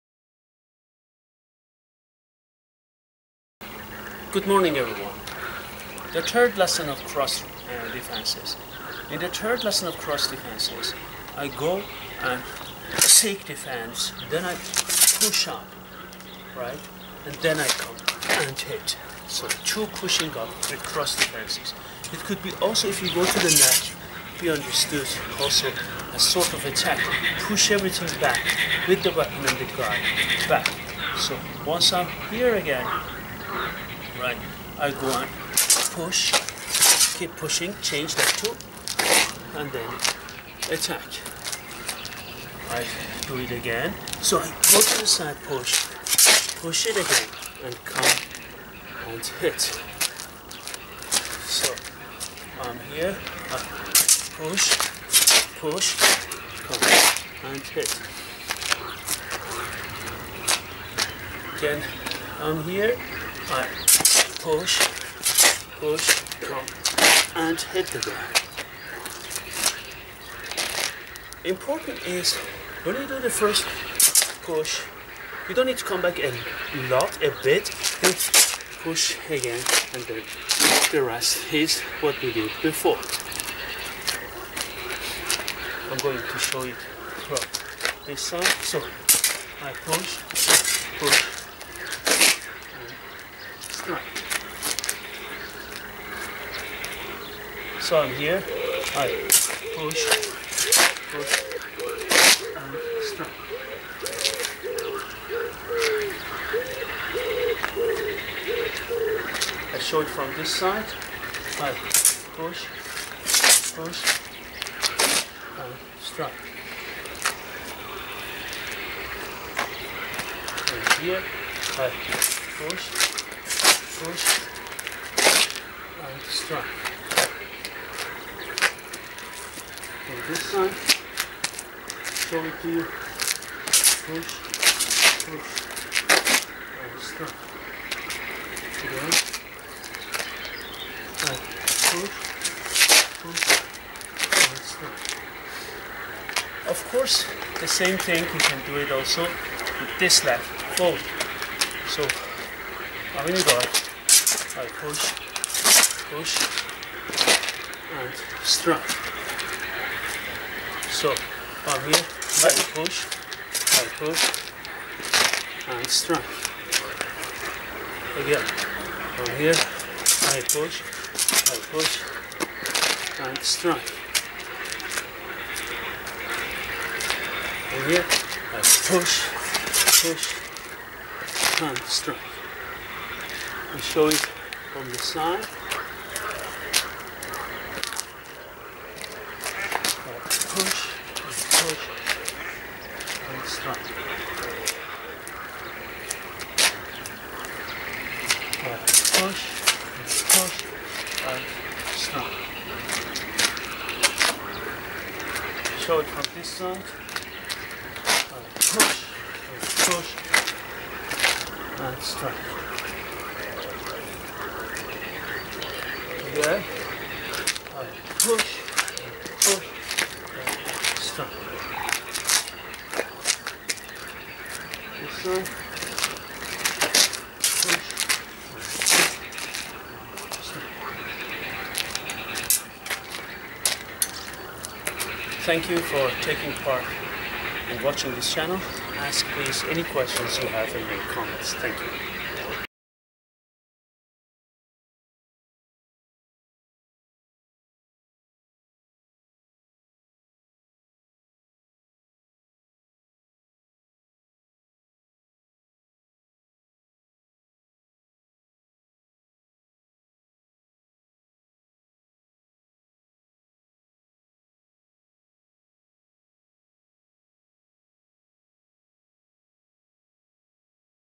Low and high cross defenses with dual shamshirs: Part 3
The following video shows how to defend with two shamshirs into cross positions